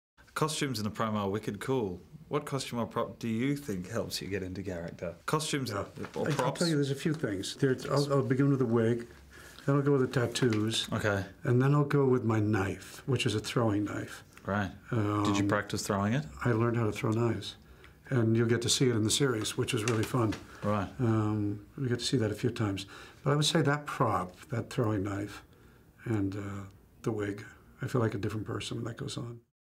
Ask Outsiders: Thomas M. Wright and David Morse discuss costumes and props
WGN America's Outsiders airs Tuesdays 9/8c.